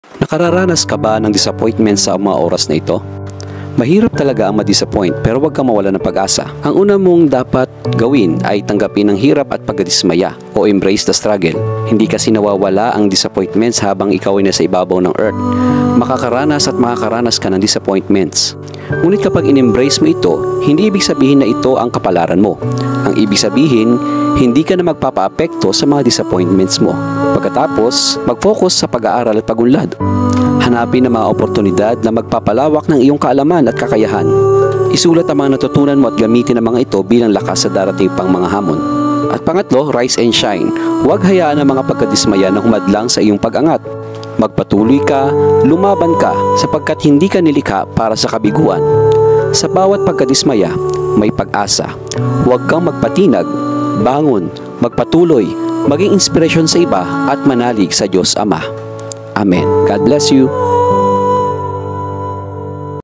PAANO MAPAGTATAGUMPAYAN ANG DISAPPOINTMENTS?
Lahat ng tao ay nakakaranas ng oagka dismaya dahil sa mga iba't-ibang mga karanasan sa buhay.

Huwag nating hayaang malugmok lang tayo sa pagkadismaya or maging disappointed forever.

Magagawa mo itong ma- overcome.

In this video, I will share 3 simple steps in overcoming Disappointments.